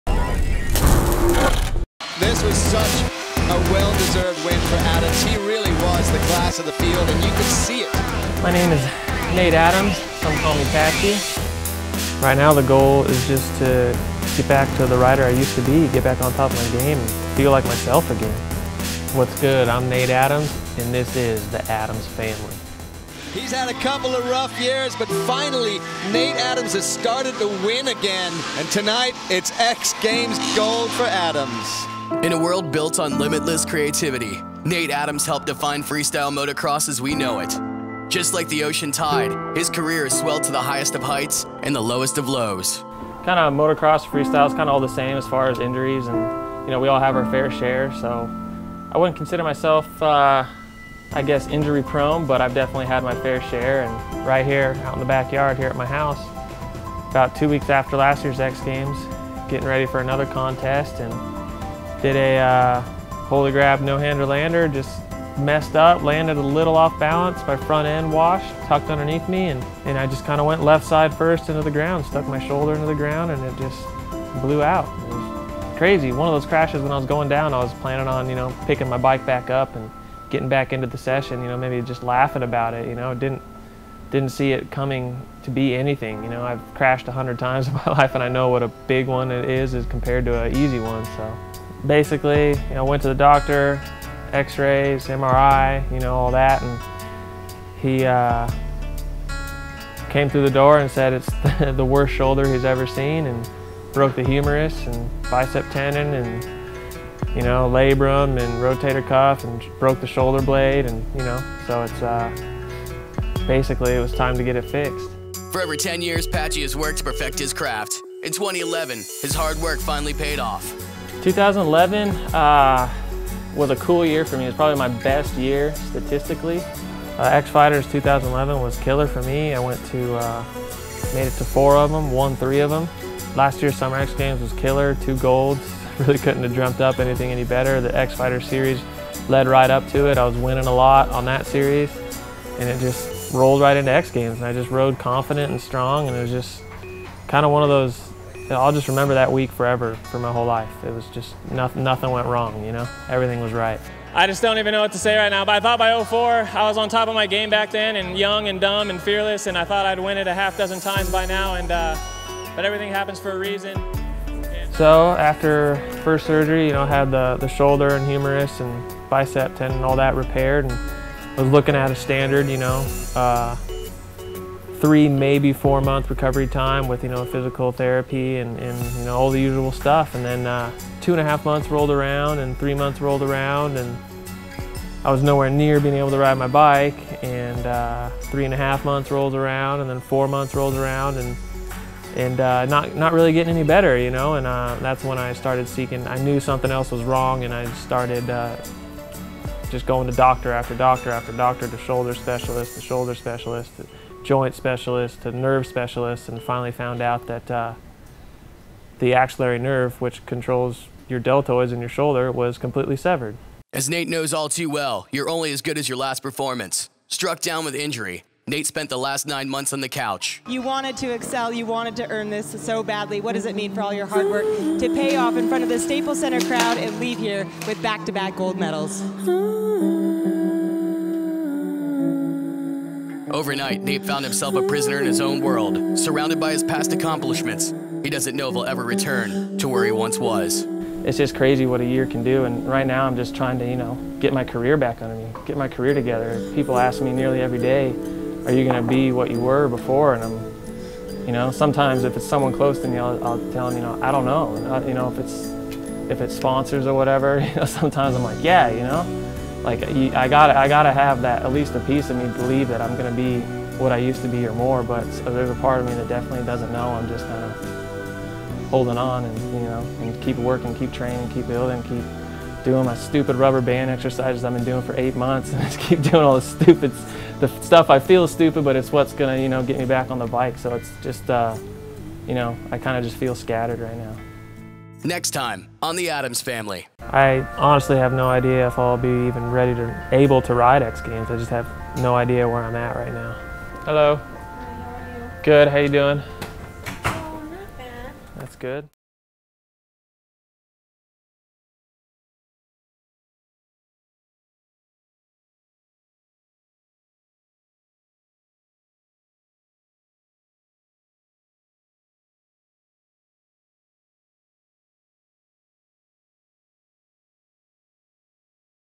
Nate Adams On The Long Road Back To X Games Moto Gold
In 2011, Nate Adams was at the top of his considerable freestyle motocross game. But with three Red Bull X-Fighters wins, two X Games golds, and a near-certain X-Fighters title in reach, an innocuous-seeming practice crash obliterated his shoulder. Eight months later, Adams is struggling to prepare for X Games 2012 and wondering if he'll ever regain his elite form.
Filmed and edited by Ryan Walters Films.